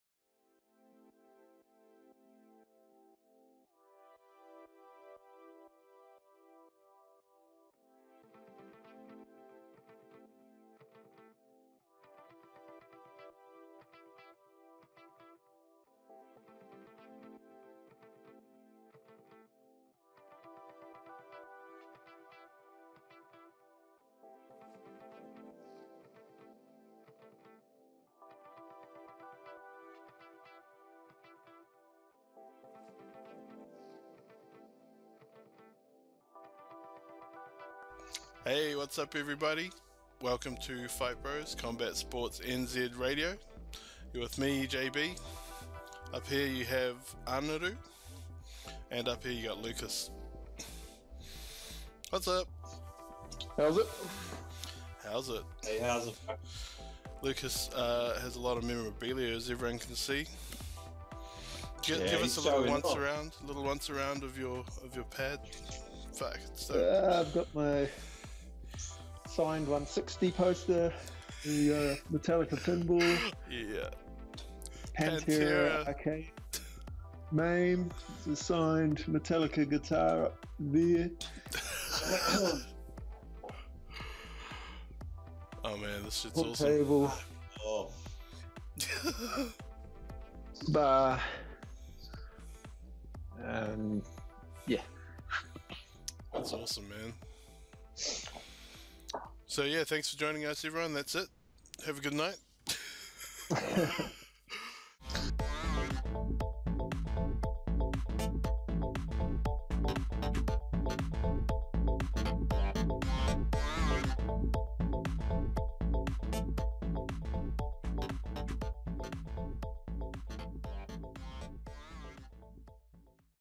FightBros Highlights - Lucas's UFC Shrine
Fight Bros co-host Lucas gives us a once around his man-cave AKA the UFC fans dream _3